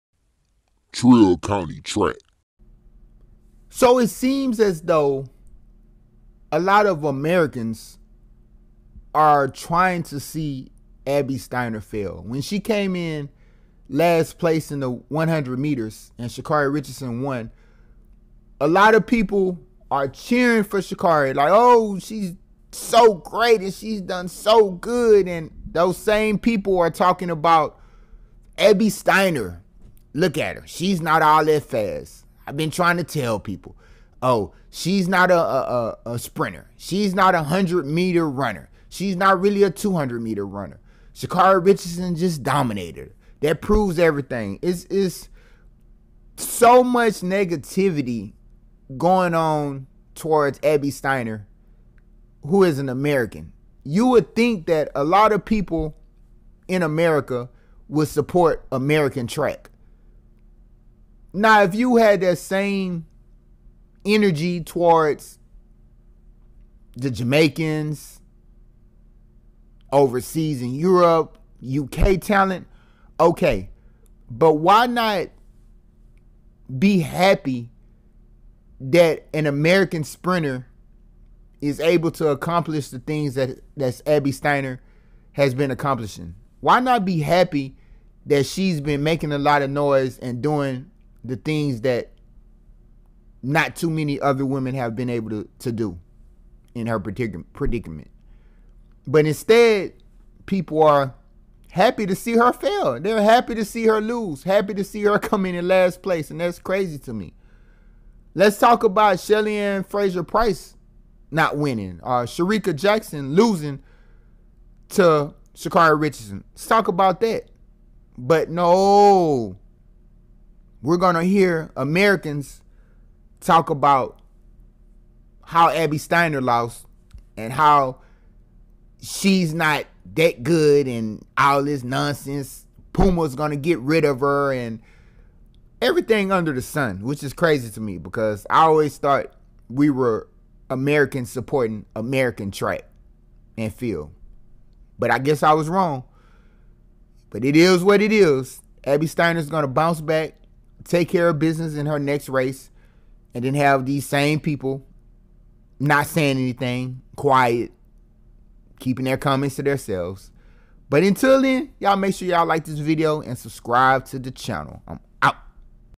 Some Americans Want To See Abby Steiner Fail ‼️ I Wonder Why 🤔
It seems like some Americans don't want to see abby steiner succeed

But she is a American and every American should support her